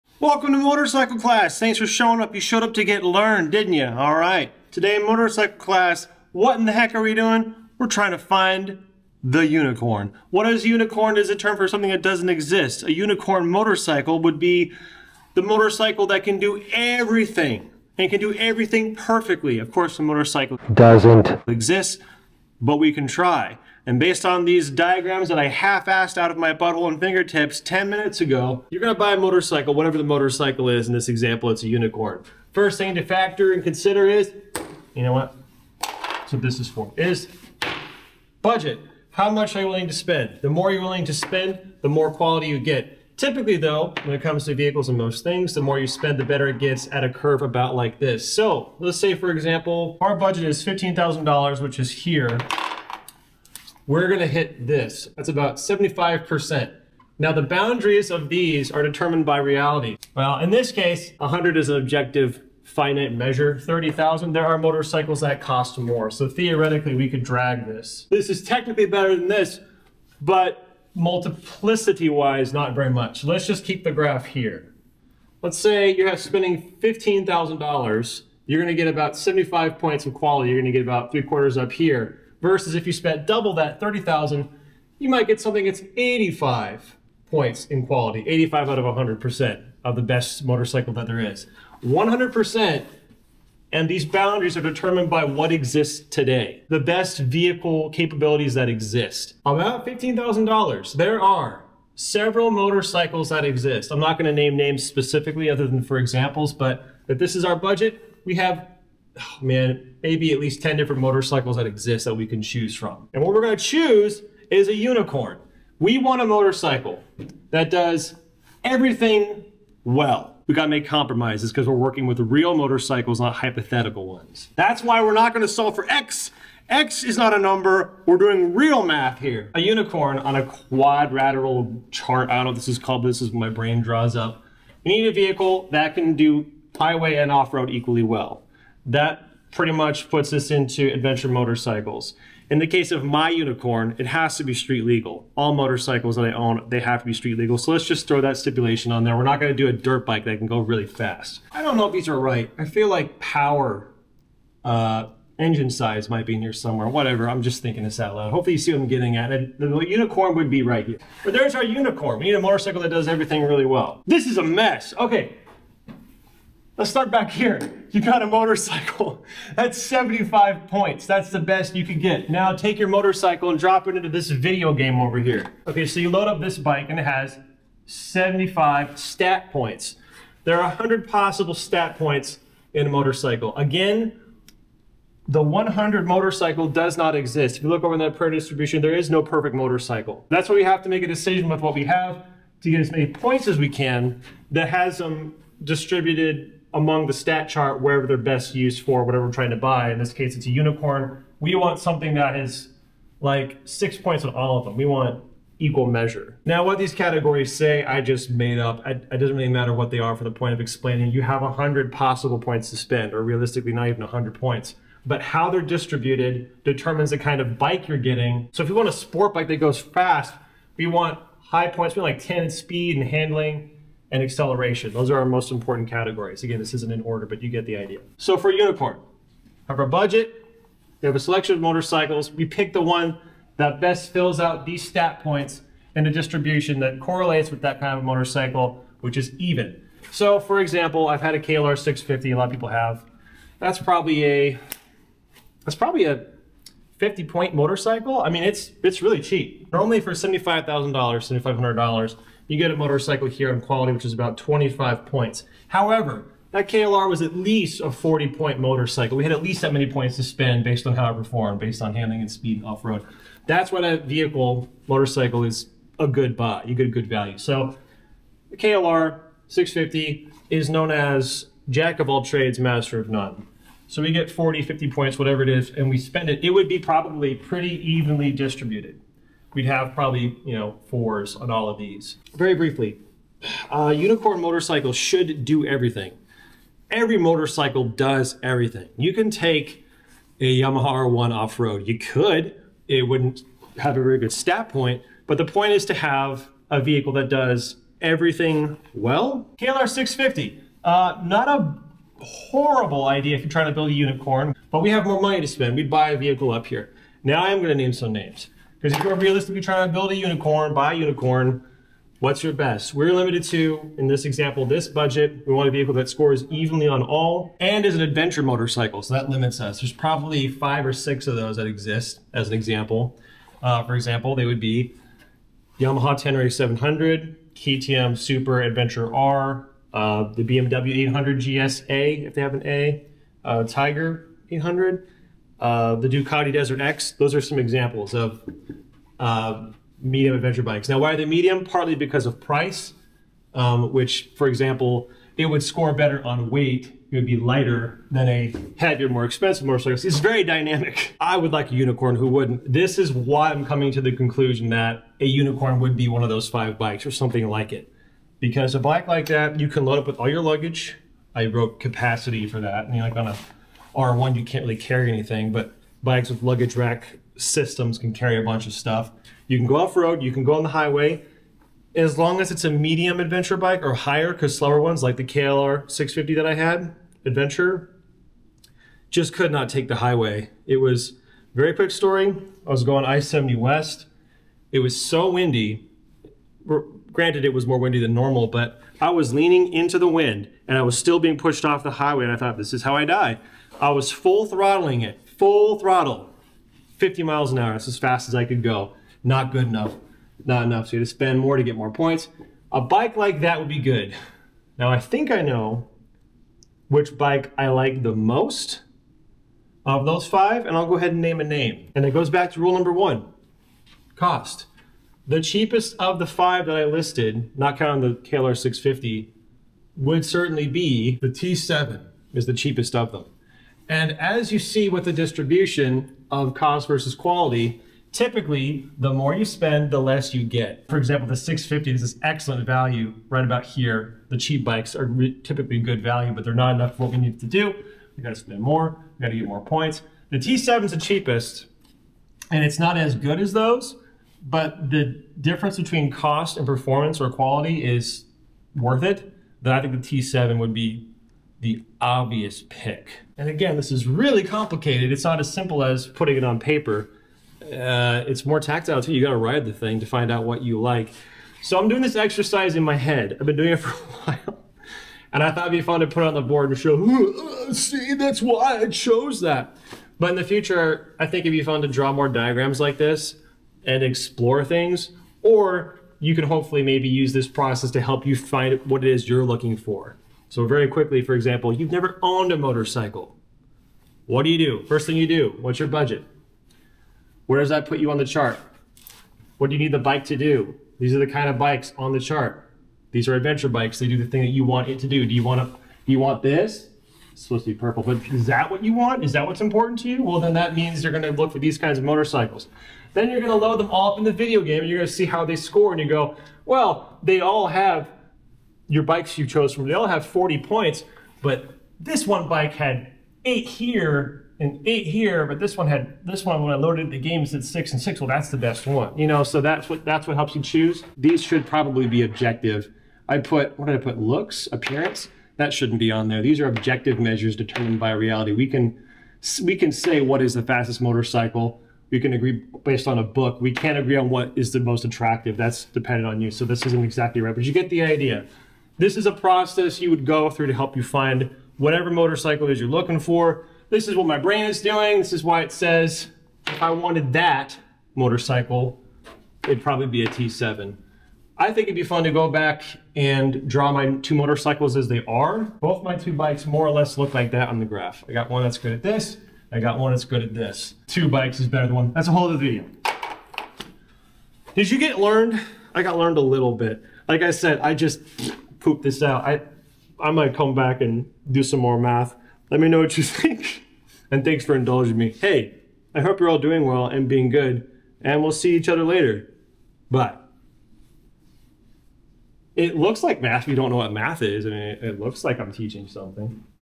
Unicorn Motorcycle Math
How do you calculate what motorcycle to get? I don't know, but it probably isn't what's in this video.